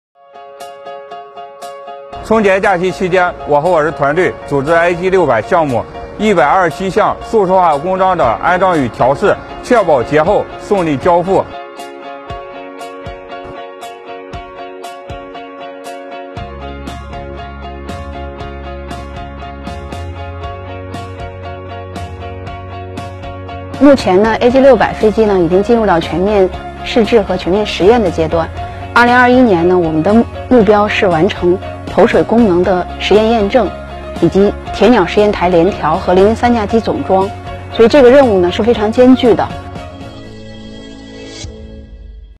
Китайская корпорация AVIC представила комплексный тренажер самолета-амфибии AG600
Китайская авиастроительная корпорация AVIC представила комплексный тренажер экипажа самолета-амфибии AG600.
Ранее сообщалось о том, что 28 января 2021 года на авиастроительном предприятии Shaanxi Aircraft Company в Шэньси прошла церемония начала постройки третьего самолета-амфибии AG600 Jiaolong ("Водяной дракон"). AG600 имеет длину 39,3 м, размах крыла 39 м и заявляемый максимальный взлетный вес 53,5 тонны (по ряду китайских источников - до 60 тонн), что делает его самым большим современным гидросамолетом в мире. AG600 имеет четыре турбовинтовых двигателя WJ6 взлетной мощностью по 5100 л.с. (копия советского двигателя АИ-20, производится предприятием China National South Aviation Industry Company (CNSAIC) в Чжучжоу, провинция Хунань) с шестилопастными воздушными винтами.
Было объявлено о разработке двух модификаций самолета AG600 - поисково-спасательной (способна принимать на борт до 50 человек) и пожарной (забор 12 тонн воды).